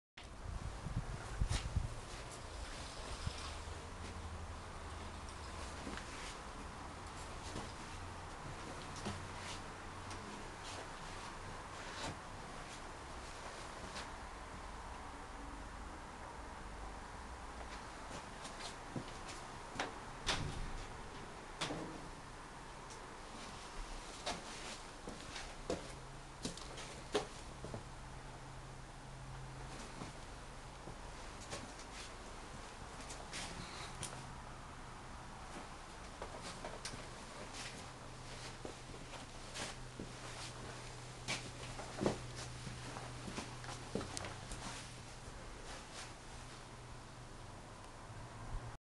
ZX9r Ninja Walkaround without engine start
a request for this video has prompted it, but its only temporary untill I make a better one with the engine running too...